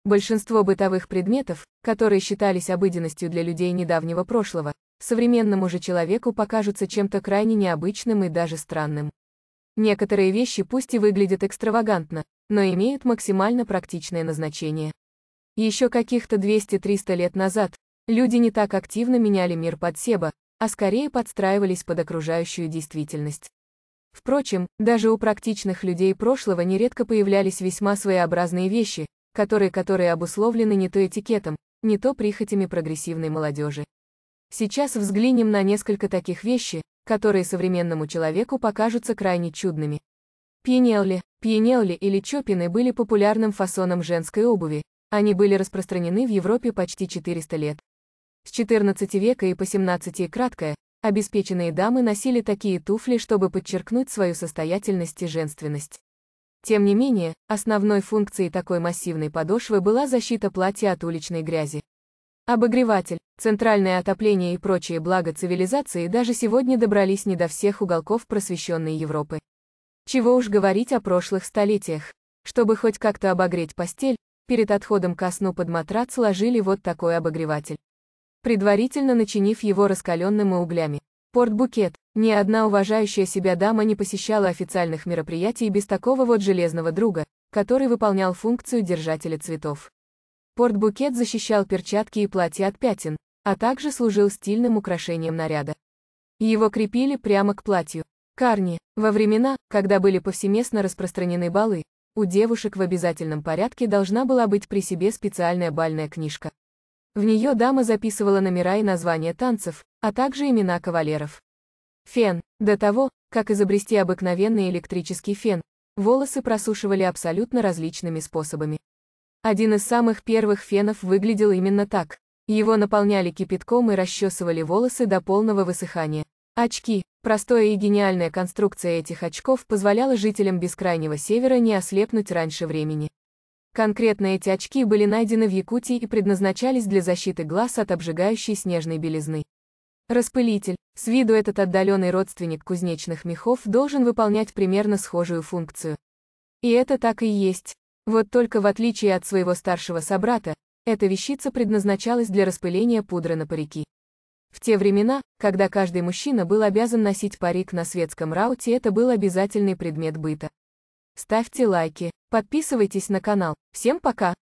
7 обычных вещей из прошлого, которые покажутся совеременному человеку очень странными
Пьянелле. Обогреватель. Портбукет. Карне. Фен. Очки. Распылитель